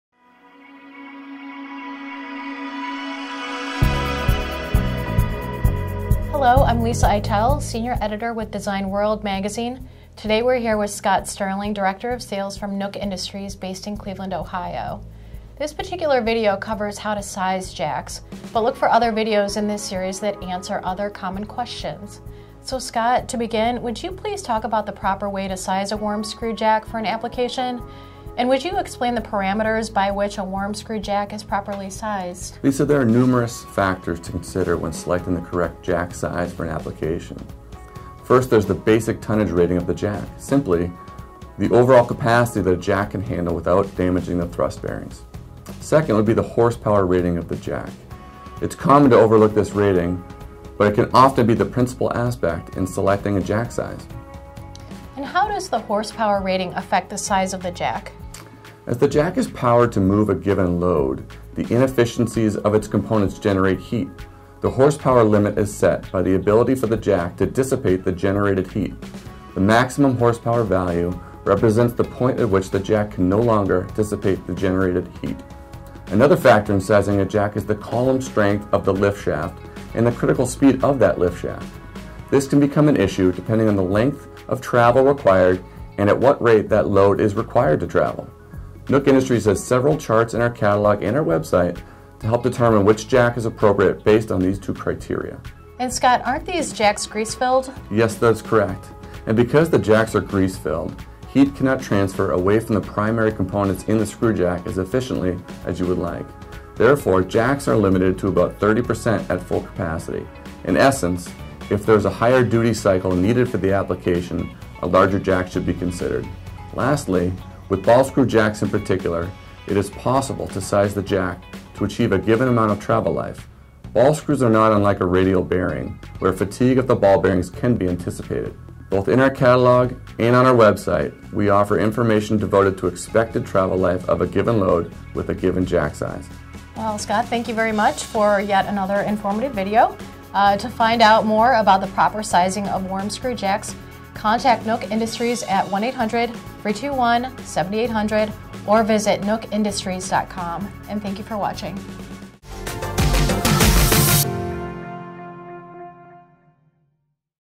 How to size a jack for industrial applications
In this video, Scott Sterling of Nook Industries based in Cleveland, talks with Lisa Eitel of Design World about the proper way to size a worm screw jack for an application. There are several criteria by which engineers can properly size worm screw jacks.